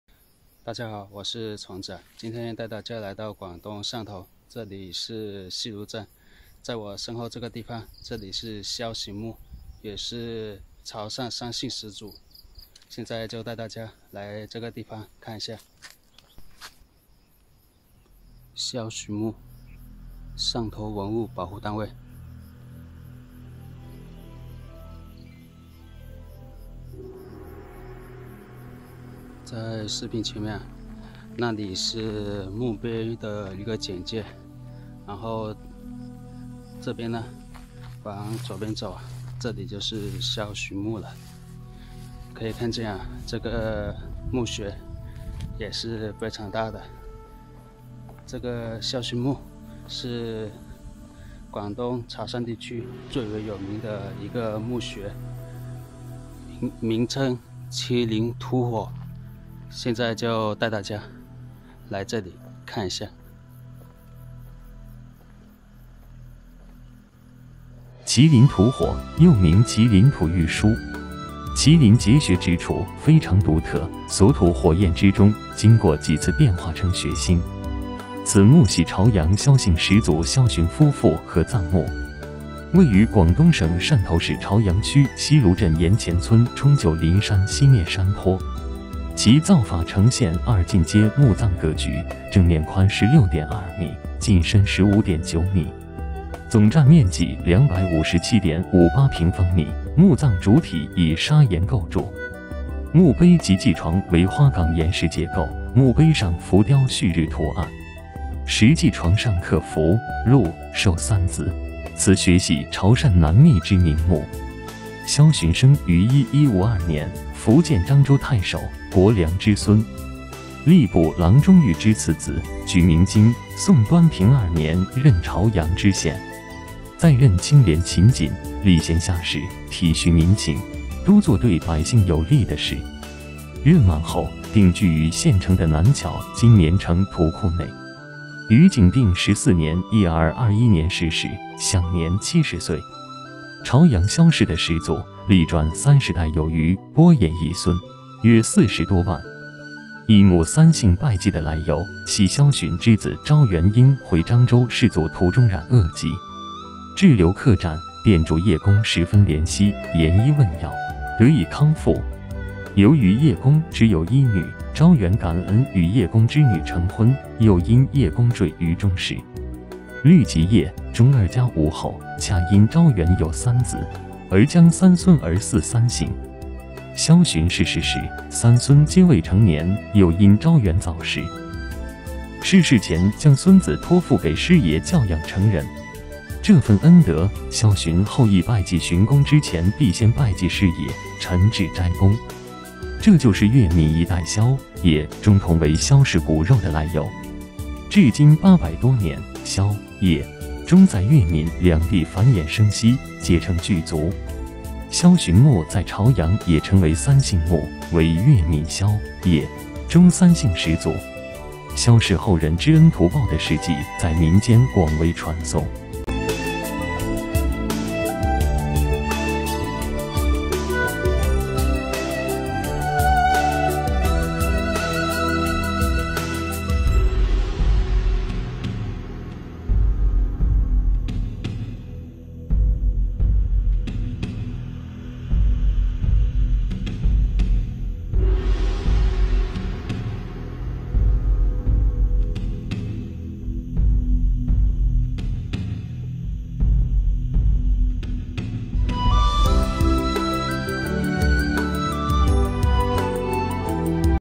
广东宋朝名墓，粤闽“萧、叶、钟”三姓共同祖先？有什么故事？【潮游记】
大家好！我是【潮游记】，
走遍潮汕古乡村，分享潮汕风景和风俗，
带你领略不一样的潮汕美景